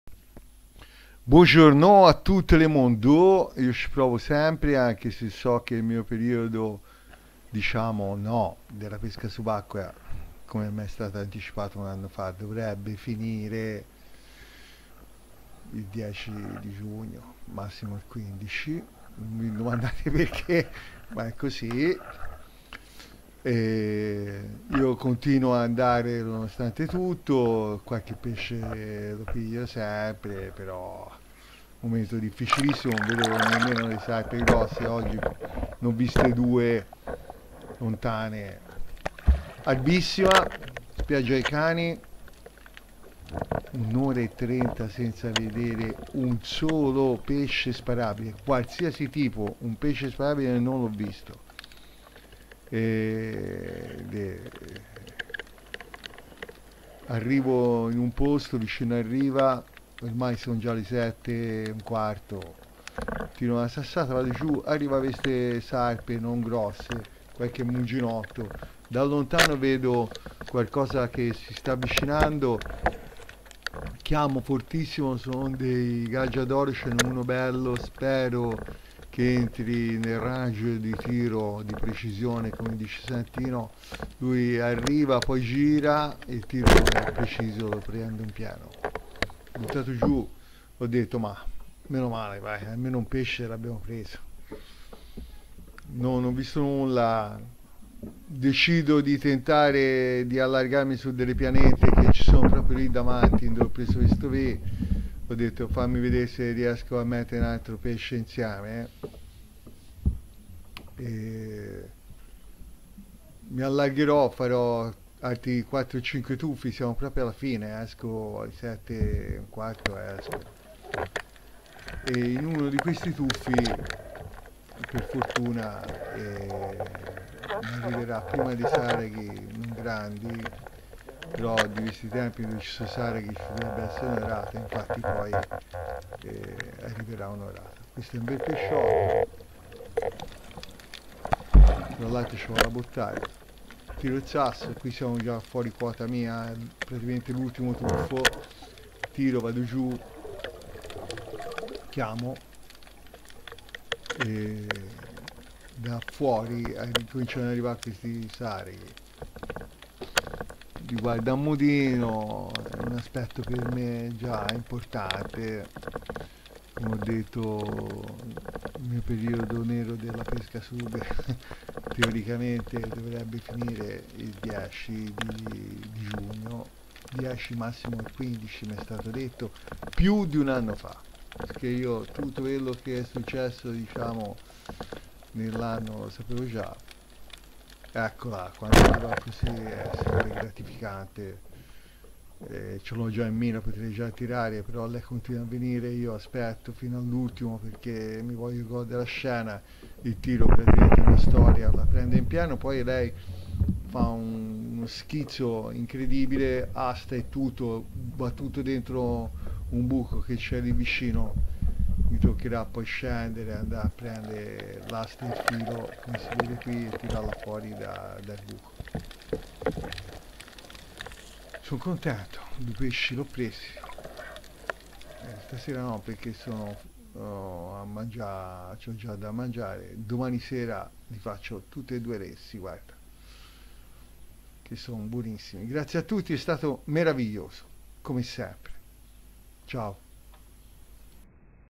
pesca sub , dai cani alla rotonda alba vera
buongiornò a tutte le mondò

vado poco in mare per rispettare l ordinanza balneare che non mi vuole fare pescare a riva , ero già a conoscenza che i primi mesi del 2022 per me per la pesca sub non sarebbero stati il massimo ma se ricordo bene la profezia diceva che il 10 giugno al massimo il 15 dovrebbe finire il periodo negativo ,

in questo periodo appena posso vado anche per meno di 2 ore per mantenere il più possibile lo stato fisico e il contatto con l acqua , ma sono veramente poche le prede che vedo e quelle che riesco a portare a tiro restando dove tocco ,

oggi due ore piene un ora e mezza non ho visto assolutamente nulla , poi un gaggia d oro subito dopo un orata , probabilmente se restavo un altra ora avrei potuto arricchire il mio carniere , ma alle 7.20 c erano già i primi bagnanti sono uscito

tutto bellissimo come sempre , del resto quando sei in mare vuol dire che va benissimo cosi